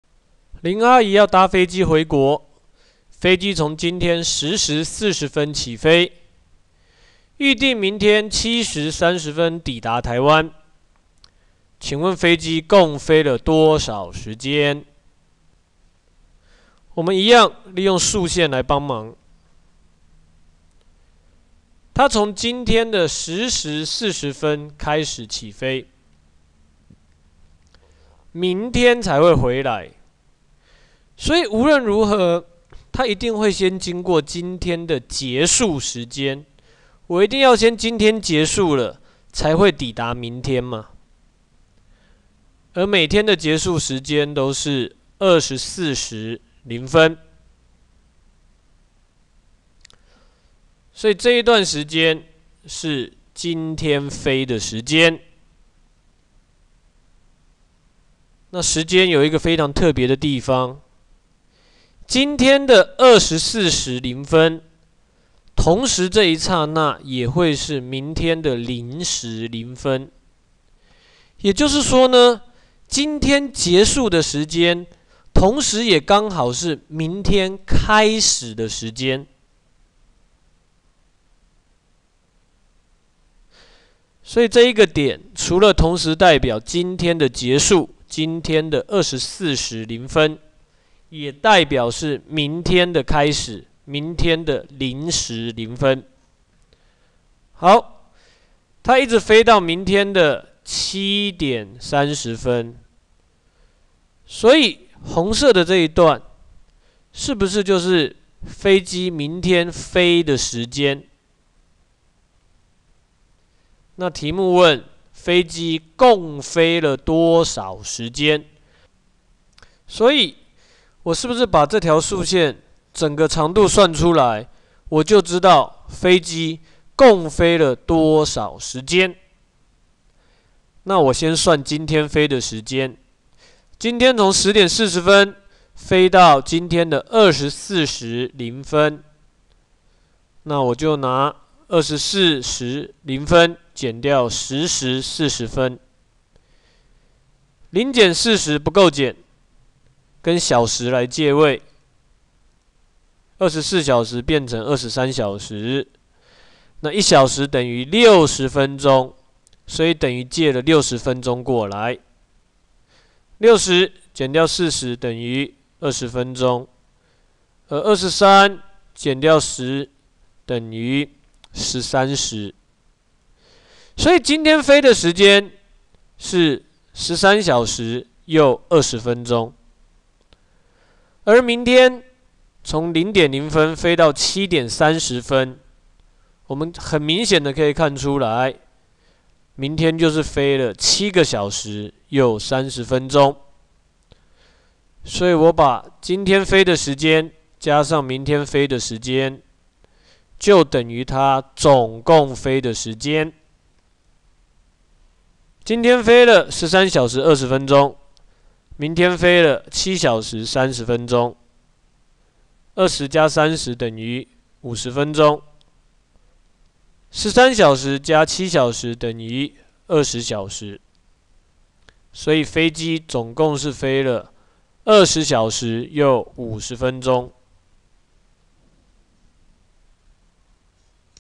時間的計算 - (03)跨日的時間計算
小學五年級數學「時間」教學，本影片在說明跨日的時間計算。